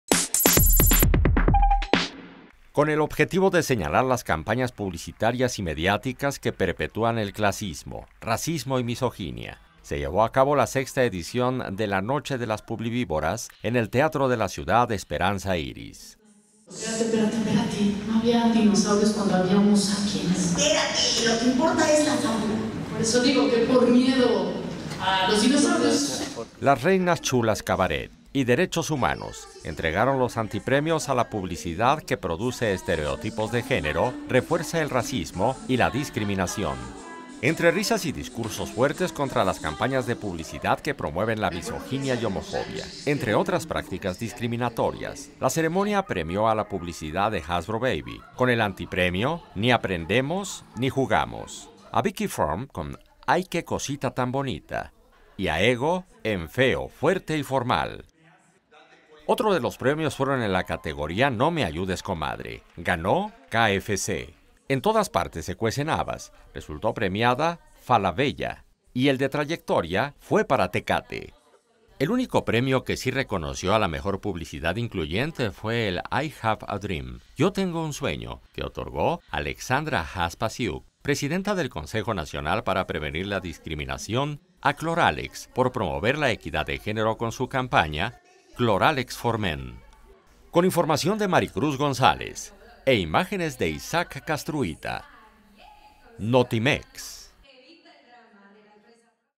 Las Publivíboras celebran su sexta noche de “antipremios"
Se premiaron las campañas publicitarias que perpetúan el clasismo, racismo y misoginia; asimismo Conapred entregó el premio "Yo tengo un sueño" a la publicidad incluyente